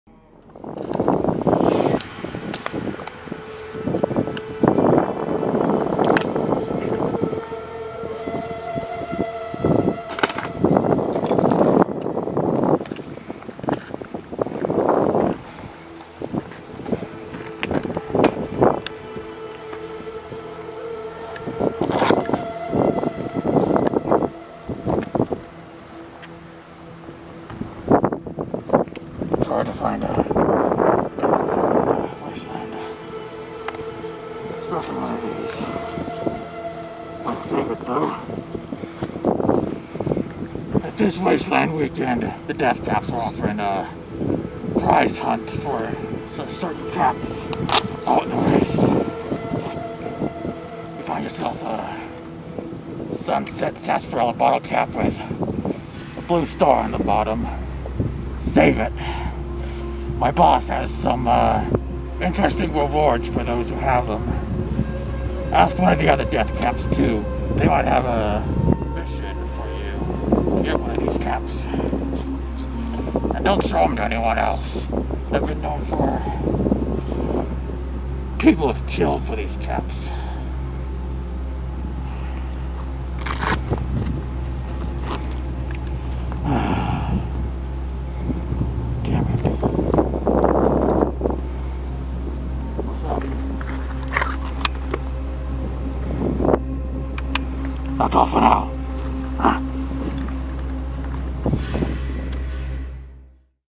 Deathcaps of the Wasteland - Star Cap Hunt
Mercenary of the Deathcaps Mulluns goes over some small details of the Blue Star Cap hunt happening this year at Wasteland Weekend 2015.